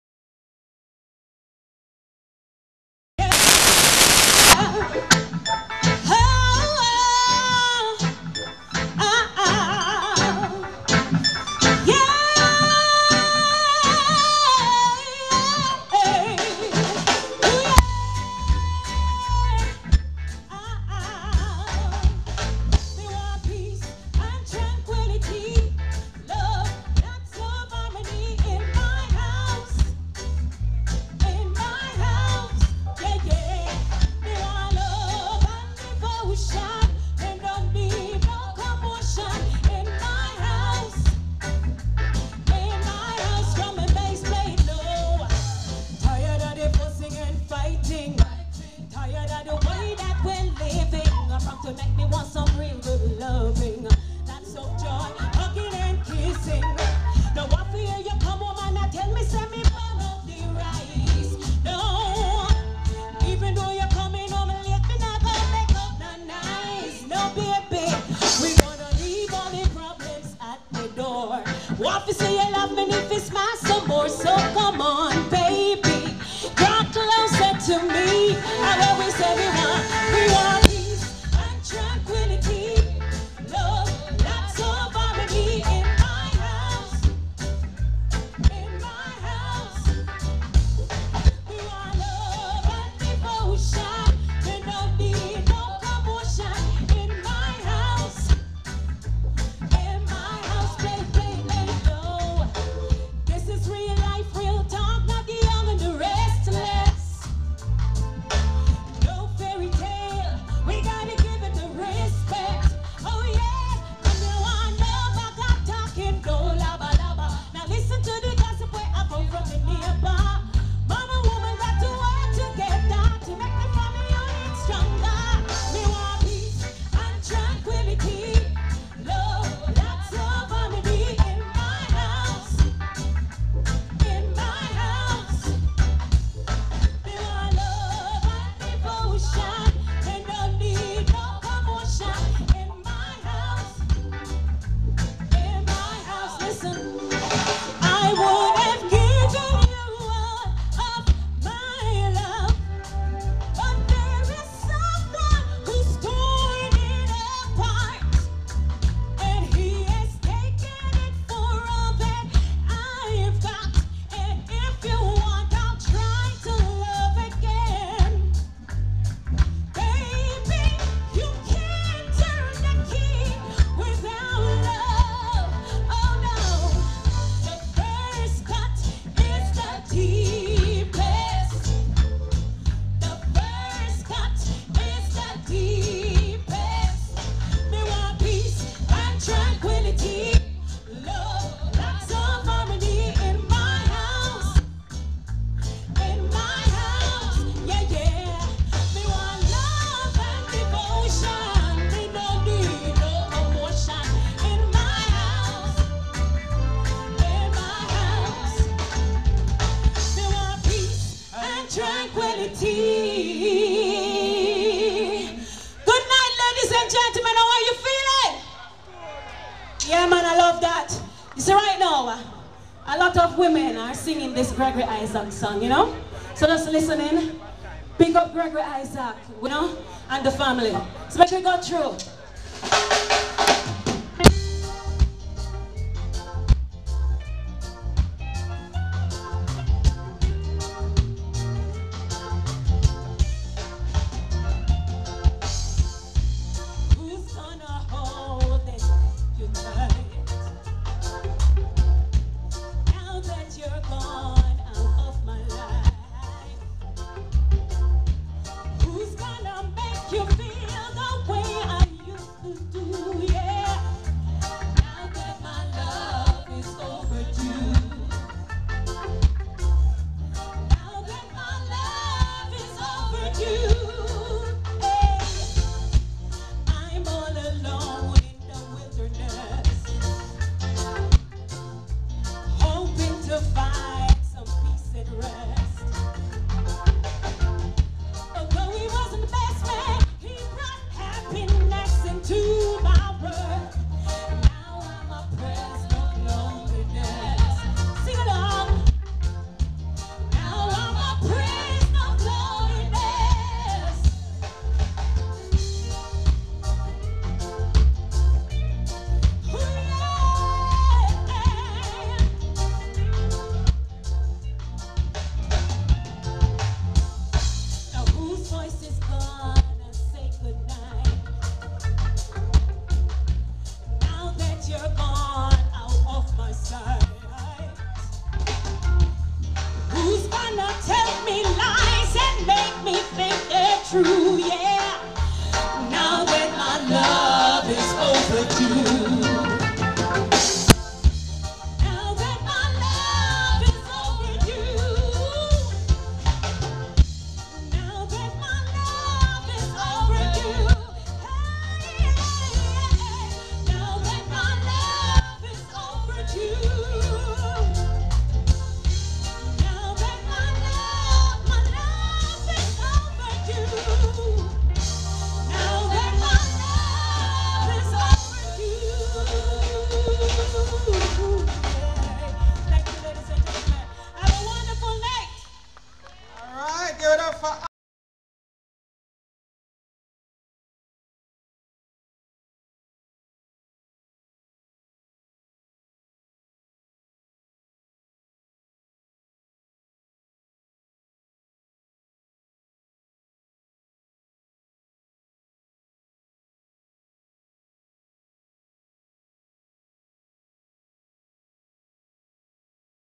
Isha bel performance @gregory Isaacs tribute Hat affair
Isha bel appeared on Gregory Isaacs tribute-"Hat affair"..July 27th 2014